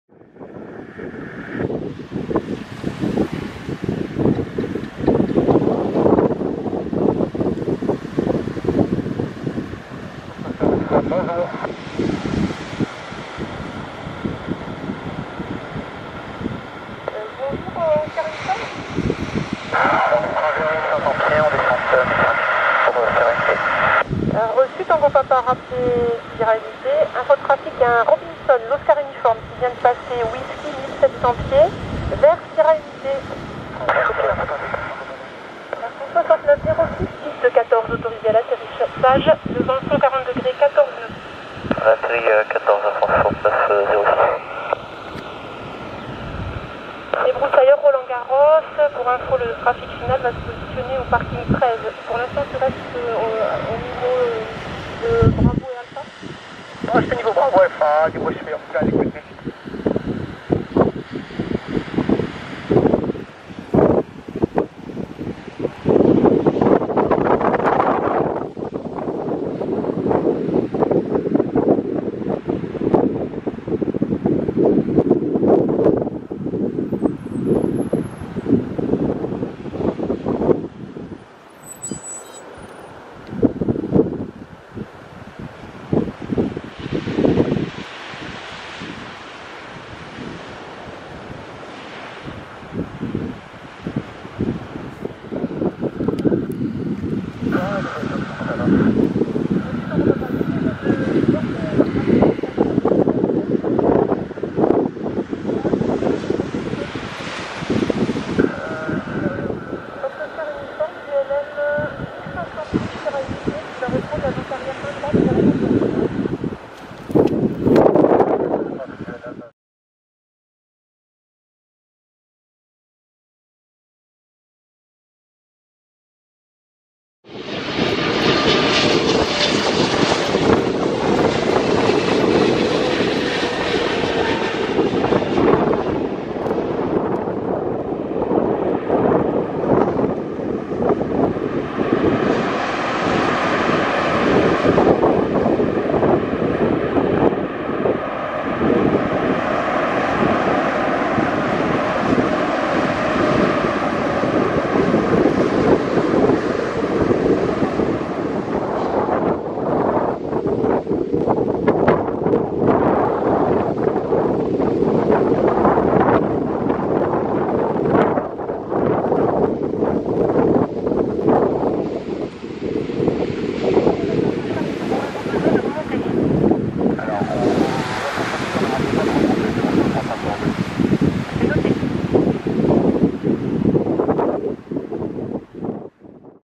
Arrivée à St Denis de la Réunion ,Boeing 777-328(ER) Air France .
Arrivée du vol cargo Air France 6906 avec le F-GSQD ,de Paris Charles de Gaulle ( CDG )